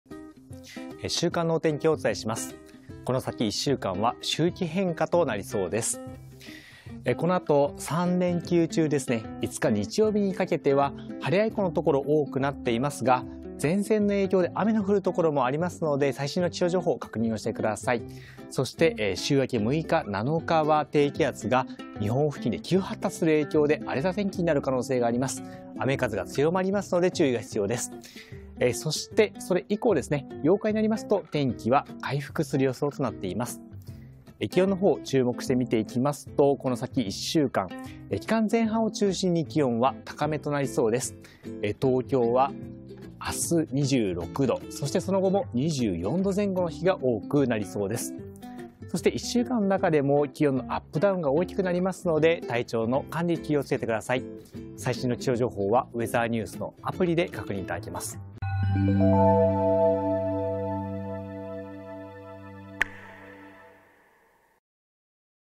【週間天気】三連休明けは前線通過 広範囲で風雨強まる
三連休中は行楽日和のところが多くなりますが、連休明けの6日(月)〜7日(火)は広範囲で天気が崩れる予想です。